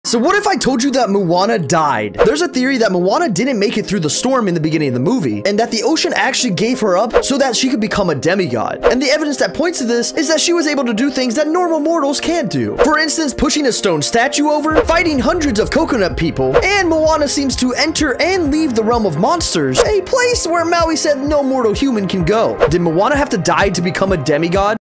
You Missed This Moana Death Theory!
Did you know that in Moana (2016), you might have missed this theory about Moana dying!

Comment your theories!